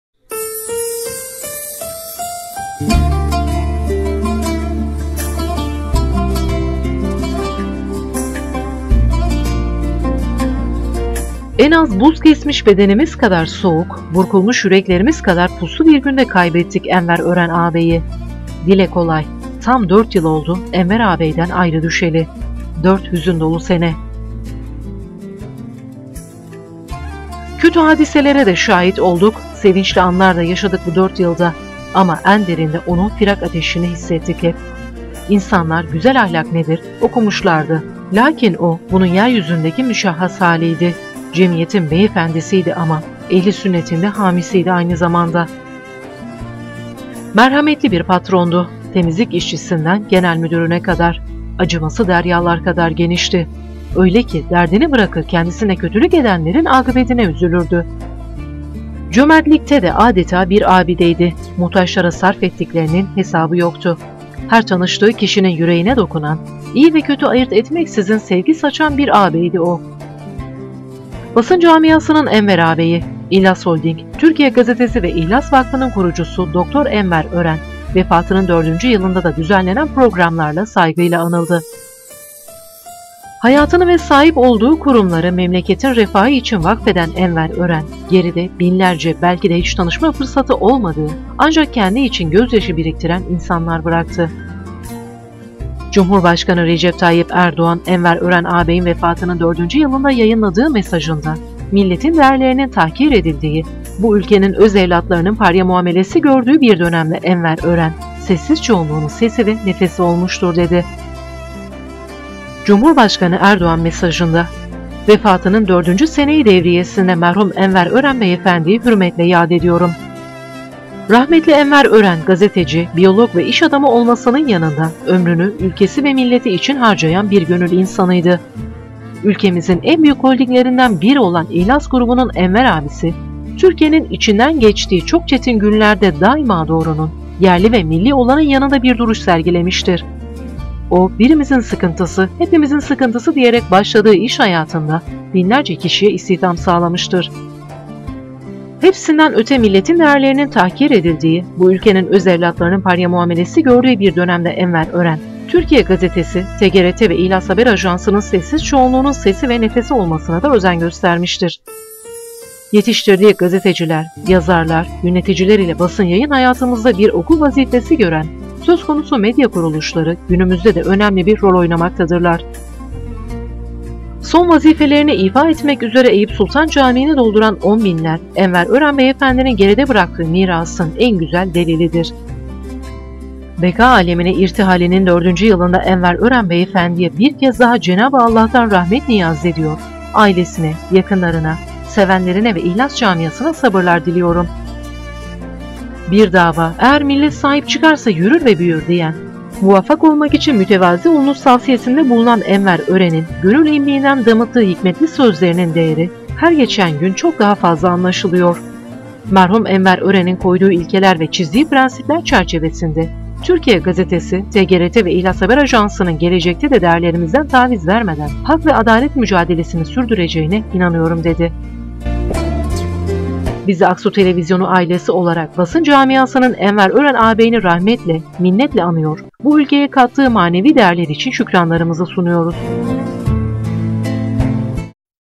Basın Camiasının Enver Ağabey'i Unutulmadı
Basın Camiasının Enver Ağabey'i İhlas Holding, Türkiye Gazetesi Ve İhlas Vakfı’nın Kurucusu Dr. Enver Ören, Vefatının Dördüncü Yılında Da Düzenlenen Programlarla Saygıyla Anıldı.